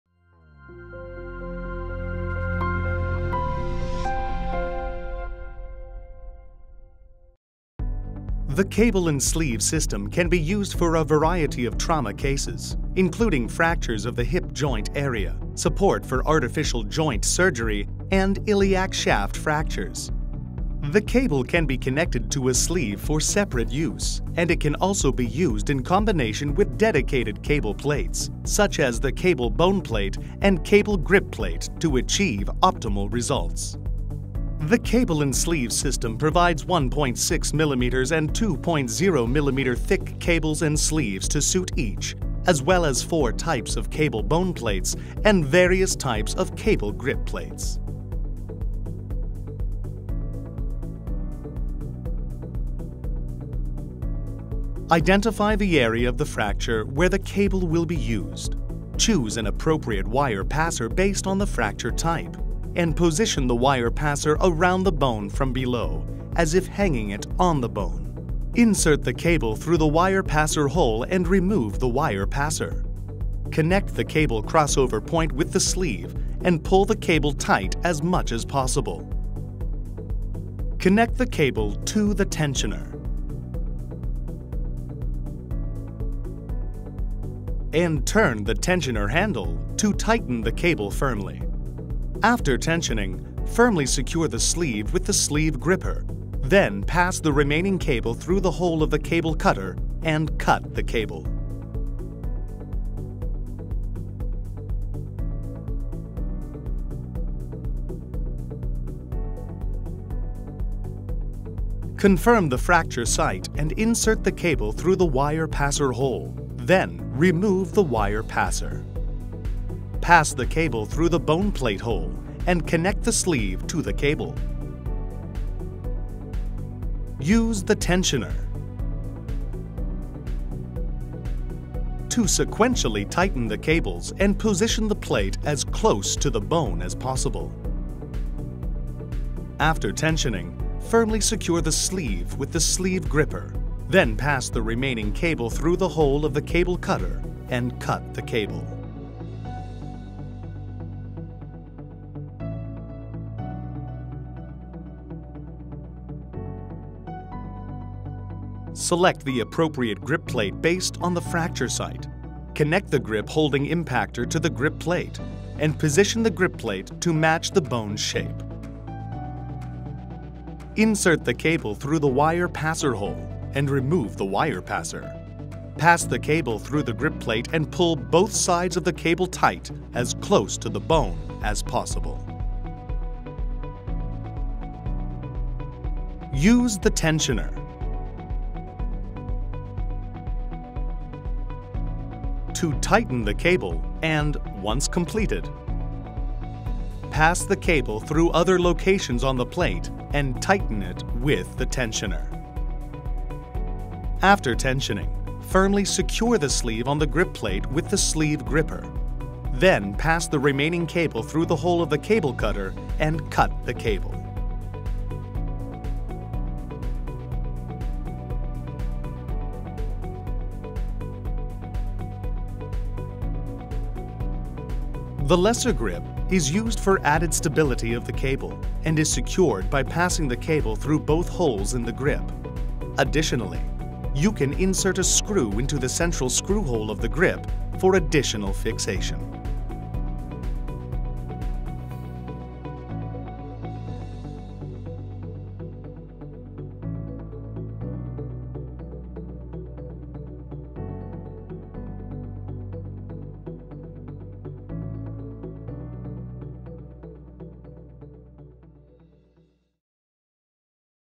[TDM Korea] Cable&Sleeve System
Welcome to the realm of innovative solutions in orthopedic surgery with the Cable&Grip System! Our system offers a wide array of bone plates and attachment devices incorporating cables for steadfast fixation.

Indications:

- Fractures in the Hip Joint Area
- Support for Artificial Joint Surgery
- Iliac Shaft Fracture

Benefits:

- Versatility: The instrumentation allows surgeons to customize tension to suit specific patient conditions.
- Security: The Cable&Grip System presents diverse bone plate and attachment options designed for secure fixation.

Plate Options:

- Short Grip Plate
- Compact Grip Plate
- Long Grip Plate
- Cable Bone Plate
- Lesser Grip (Standard/With Hook)

Cable Options:

- Cable and Sleeve Diameters of 1.6mm and 2.0mm

For further inquiries, please email the address below.

Stay up to date with the latest news and events by following us: